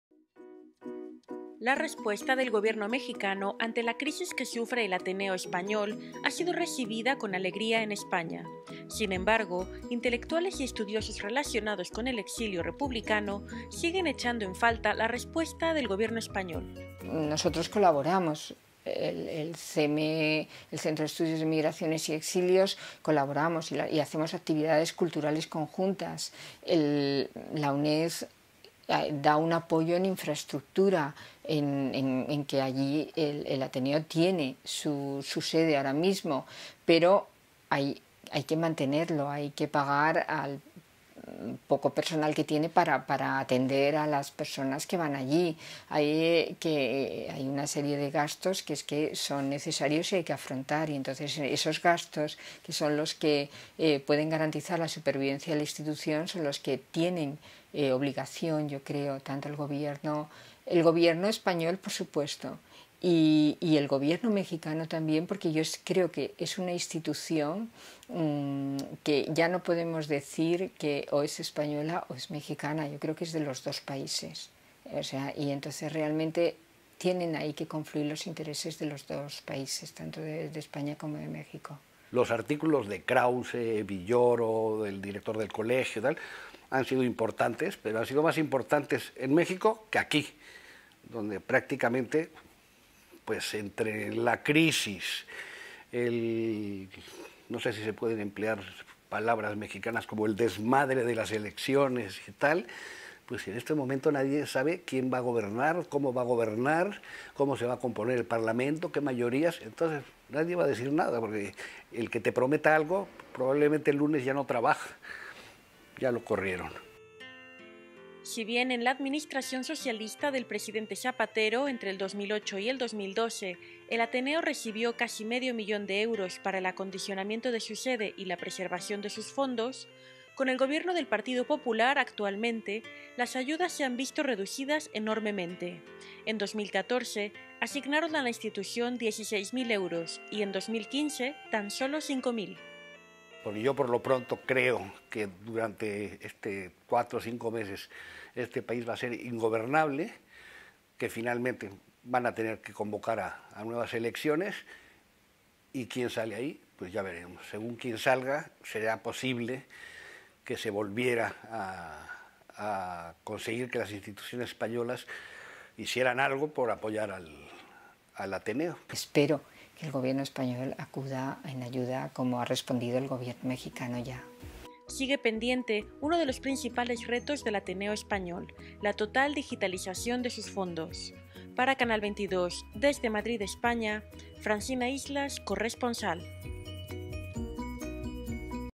La crisis del Ateneo Español de México vista desde España
Intelectuales y estudiosos cuestionan al gobierno español sobre la crisis del Ateneo Español